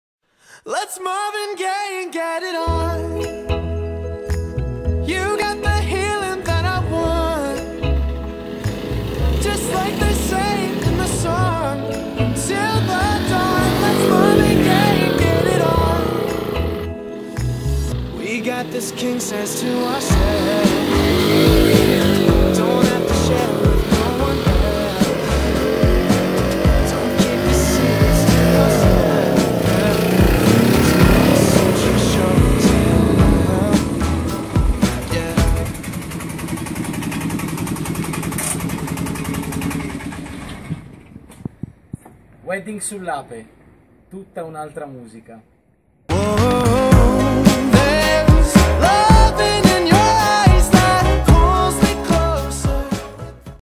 WEDDING SULL'APE - IL RODAGGIO
Sveliamo una piccola anteprima del nostro Ape#2 ..."BIANCA", dal portamento più ELEGANTE e dotata di ogni comfort per allietare i vostri APE-ritivi nuziali!
》》》 SEGUI L'APE  !  》 》 》